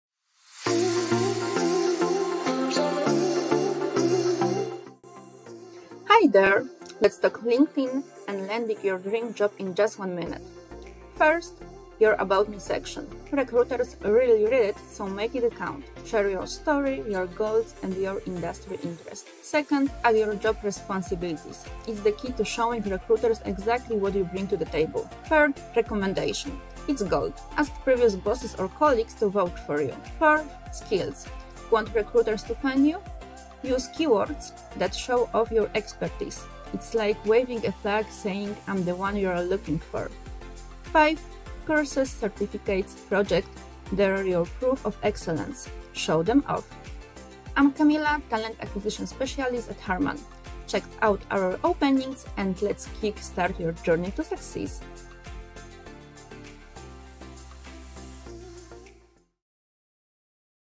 HARMAN Hiring Hacks | LinkedIn Profile
Ready to elevate your LinkedIn game and catch the attention of top recruiters? 🚀 Our Talent Acquisition Specialist at HARMAN, spills the beans on what makes a standout LinkedIn profile that recruiters can't resist.

Don't miss out on this golden opportunity to supercharge your career journey! 🎥 Take the first step towards landing your dream job at HARMAN! 💼✨